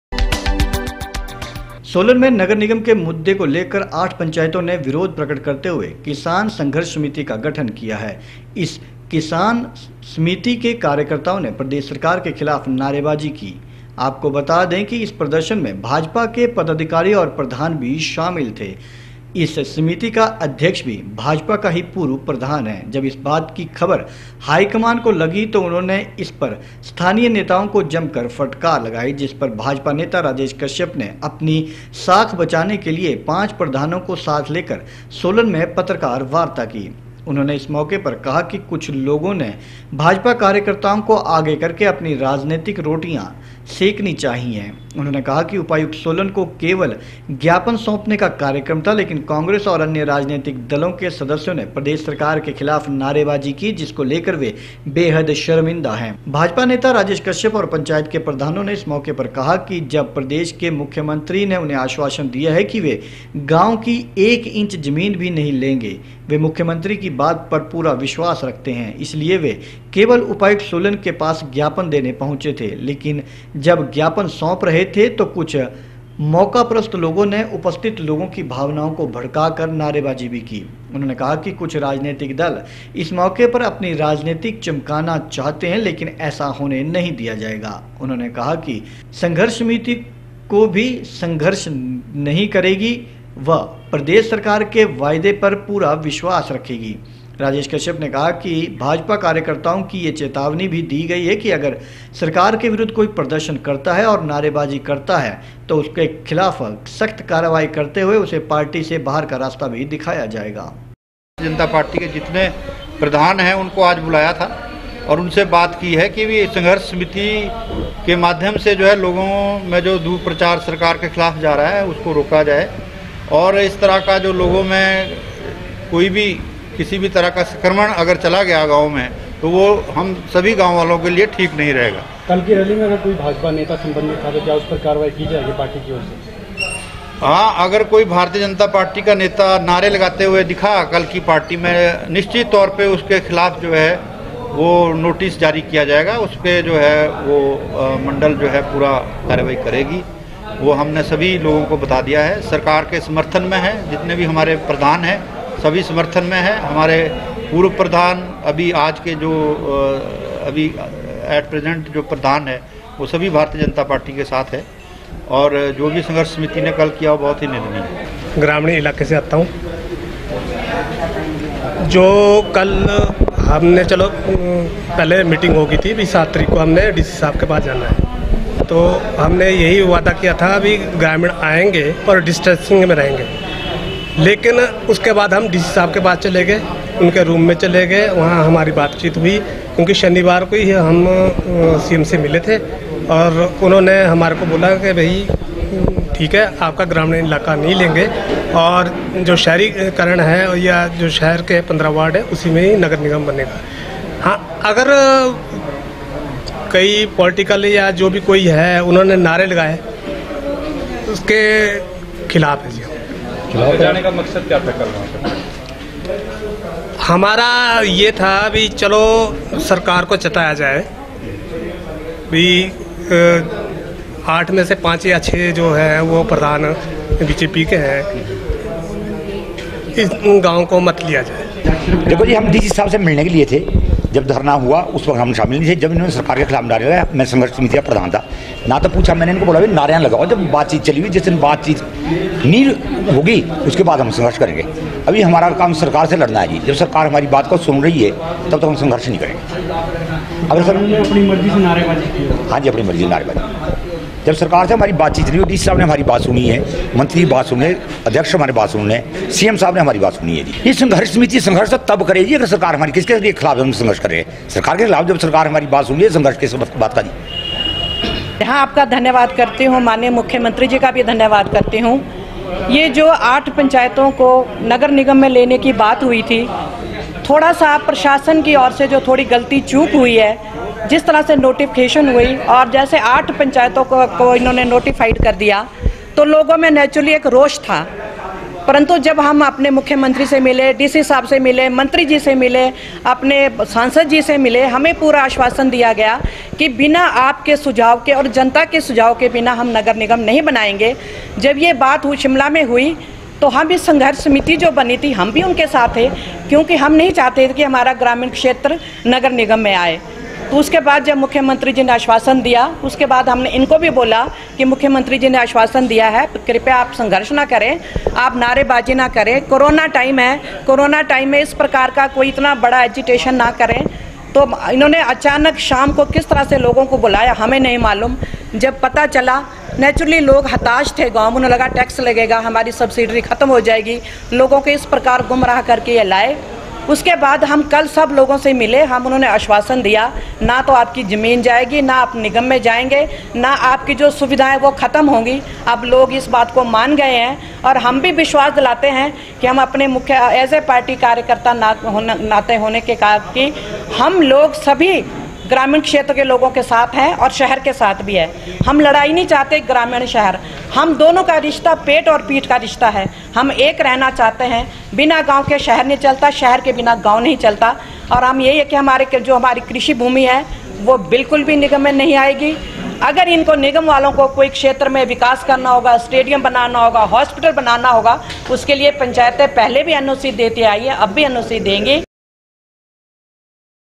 9 SEP 6 किसान समिति के कार्यकर्ताओं ने प्रदेश सरकार के खिलाफ जम कर नारे बाजी की
सोलन में नगर निगम के मुद्दे को लेकर आठ पंचायतों  ने  विरोध प्रकट करने के लिए  किसान संघर्ष समिति  का गठन किया । इस किसान समिति के कार्यकर्ताओं ने प्रदेश सरकार के खिलाफ जम कर नारे बाजी की ।मजे की बात यह रही कि इस प्रदर्शन में ,भाजपा के पदाधिकारी और प्रधान भी शामिल थे और इस समिति का अध्यक्ष भी भाजपा के  पूर्व प्रधान हैं । जब इस बात की खबर हाई कमान को लगी तो उन्होंने इस पर स्थानीय नेताओं को जम कर फटकार लगाई। जिस पर भाजपा नेता राजेश कश्यप ने अपनी साख बचाने के लिए पांच प्रधानों को साथ लेकर  सोलन में प्रेस वार्ता का आयोजन किया उनहोंने इस मौके पर कहा कि कुछ लोगों ने भाजपा कार्यकर्ताओं को आगे कर अपनी राजनितिक रोटियां सेकनी चाही उन्होंने कहा  उपायुक्त सोलन  को  केवल ज्ञापन  सौंपने का कार्यक्रम था  लेकिन कांग्रेस और अन्य राजनैतिक दलों के सदस्यों  ने प्रदेश सरकार के खिलाफ नारे बाजी की जिसको लेकर वह बेहद शर्मिंदा है |